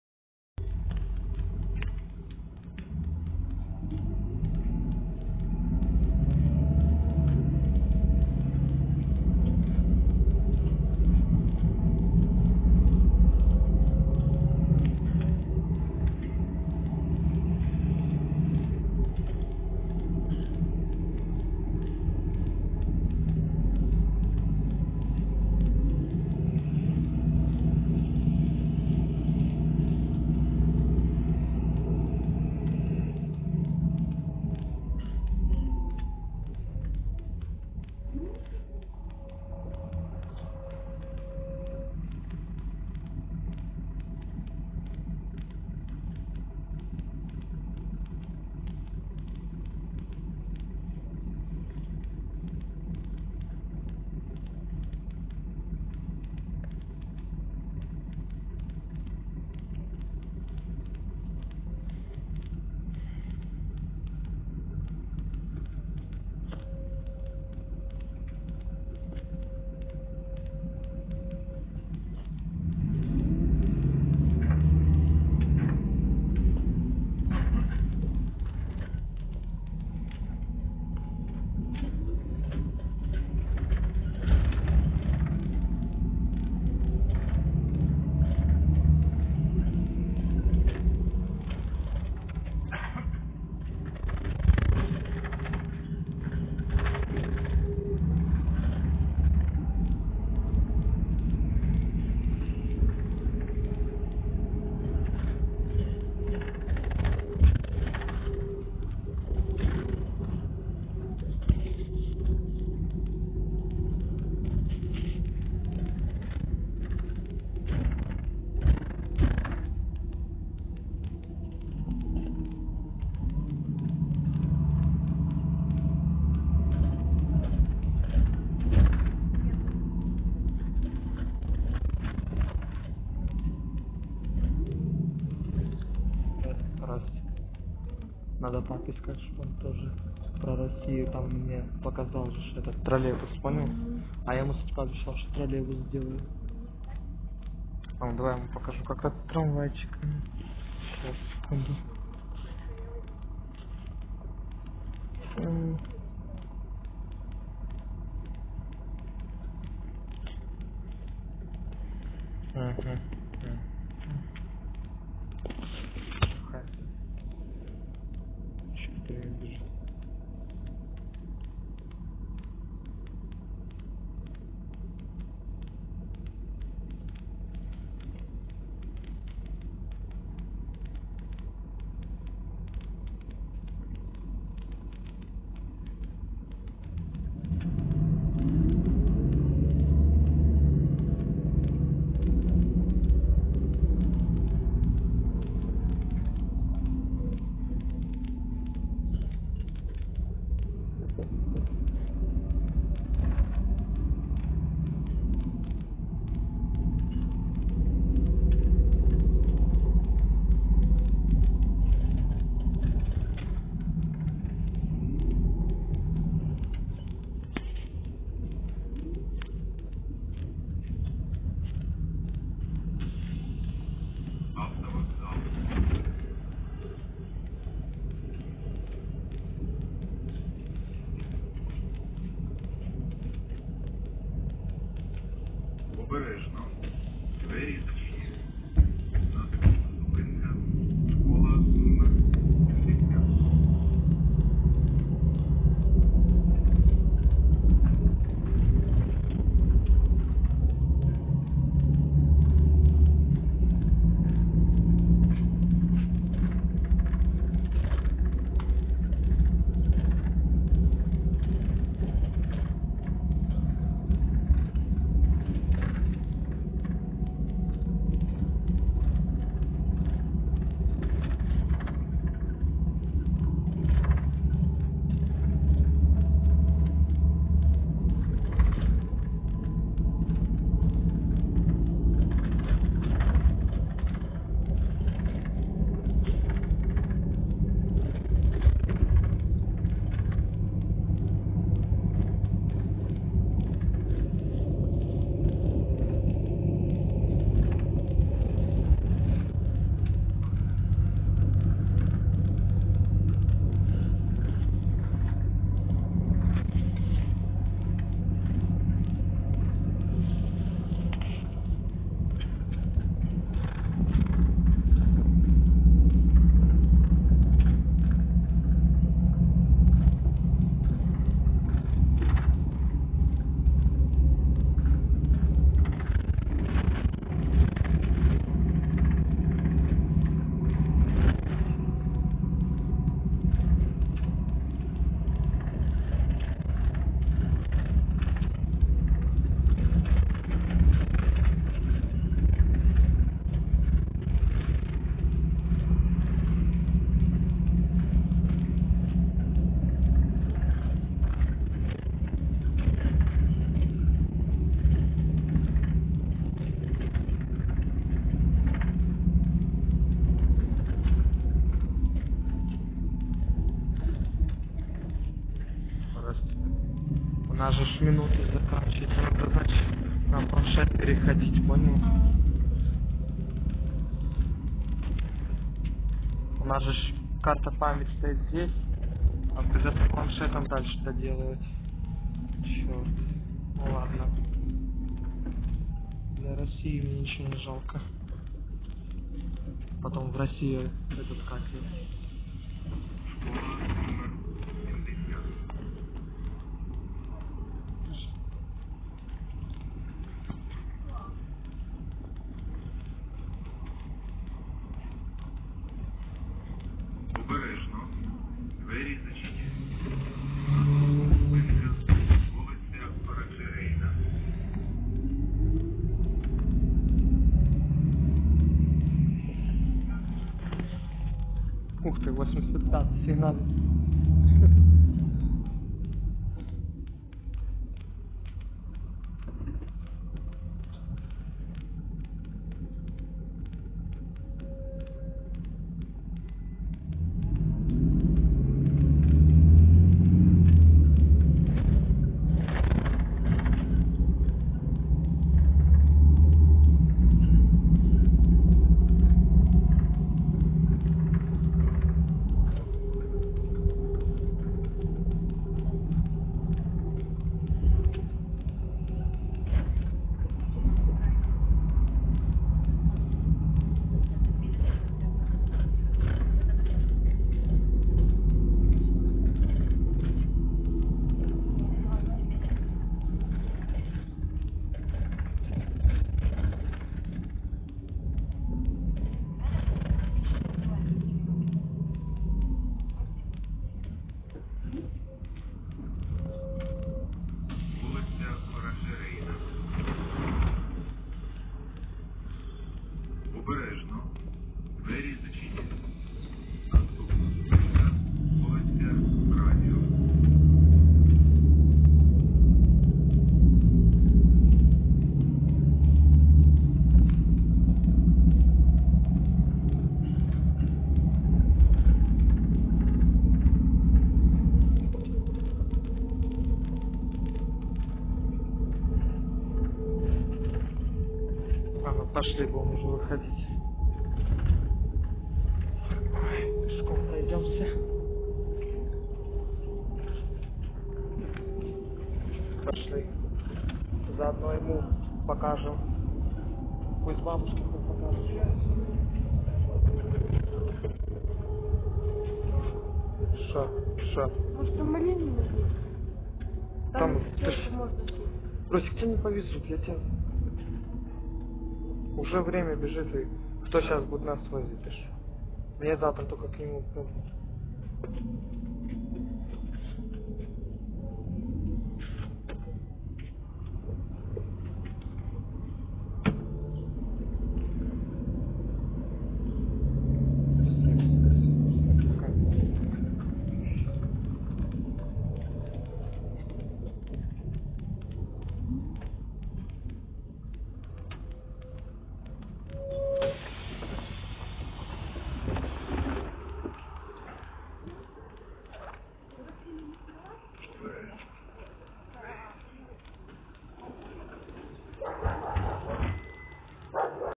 Поездка в 59 Афтобусе город Запорожье ночу
Я ехал от Автовокзал до Кинотеатр Космос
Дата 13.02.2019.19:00